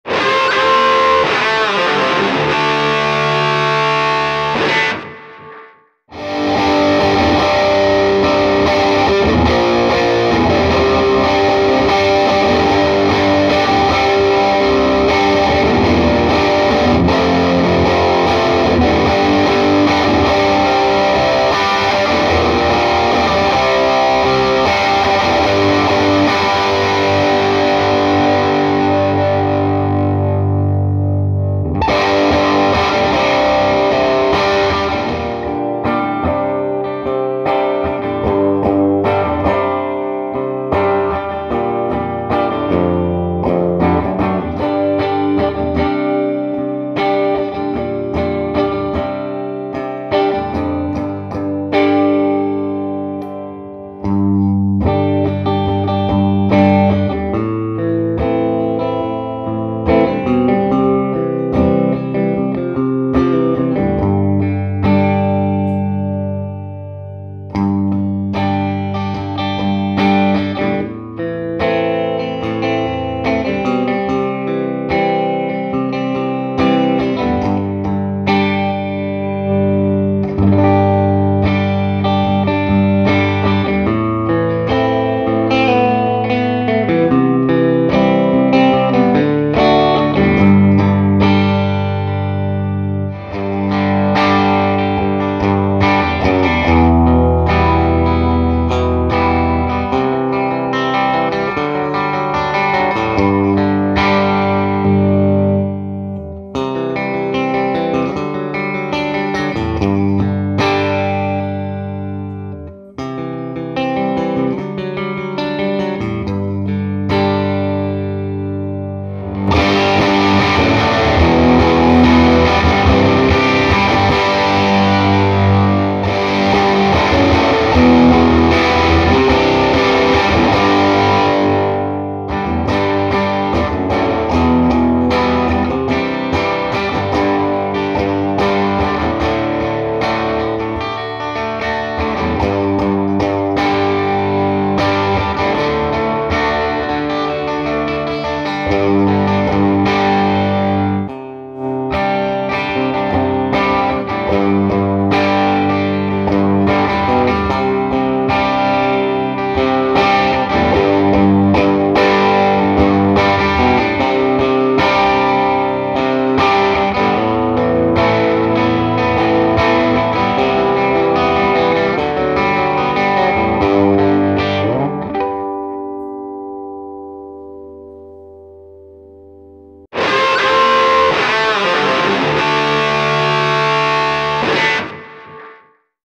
Bartel Sugarland amp demo.
No shredding here, just open chords on a classic guitar into this incredibly dynamic amp.
Signal chain: 1995 Gibson Custom Shop R9 - Bartel Sugarland.